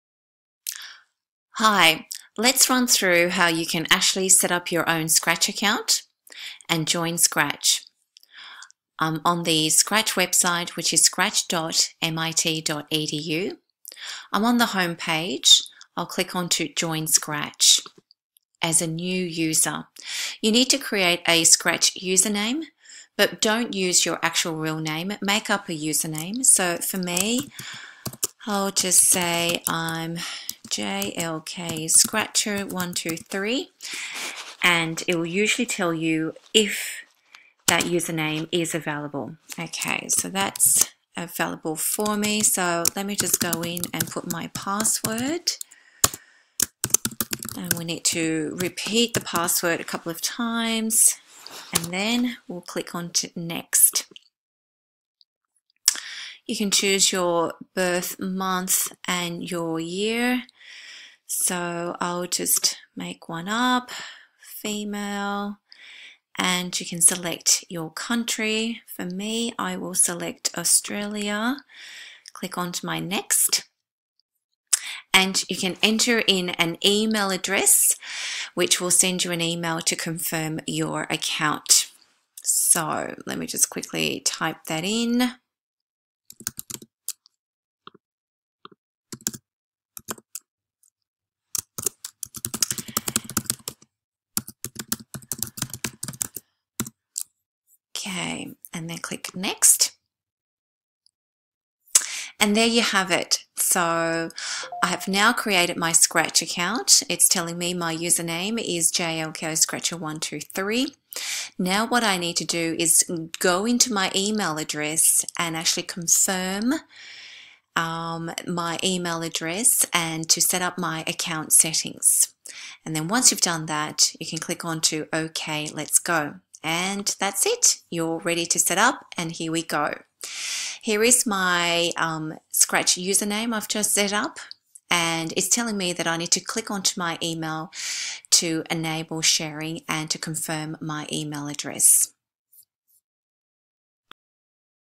Scratch Basics: How to set up a Scratch account?
This is a quick video explaining how you can set up a Scratch account.